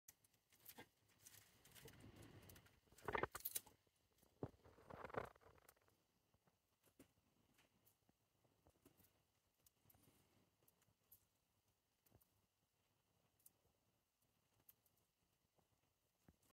Blue tongue skink eating cucumber
Powerful jaws. That piece of cucumber never stood a chance.